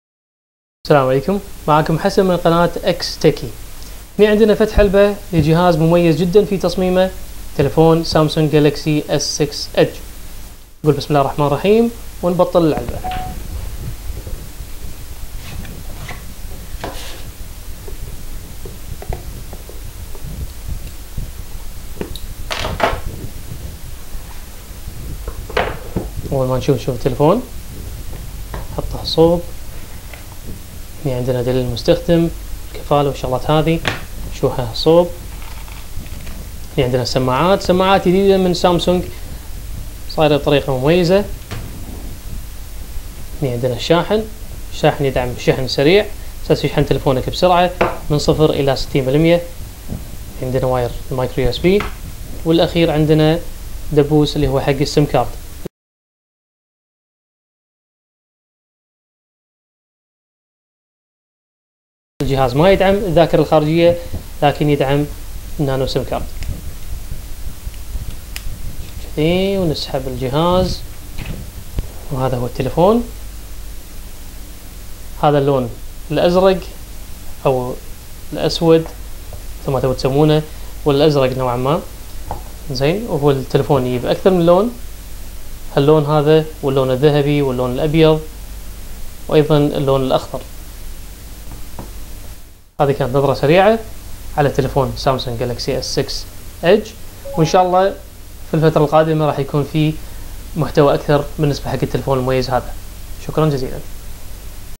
فتح علبة و نظرة اولية سامسونج جالكسي اس 6 ايدج
Unboxing and first look for the Samsung Galaxy S6 Edge
هاتف مميز جدا من شركة سامسونج يتمتع بتصميم فريد
شاشة منحنية من الاطراف بشكل انيق

الهاتف يتوفر باكثر من لون: الازرق, الابيض, الذهبي و الاخضر
هنا نرى الهاتف الازرق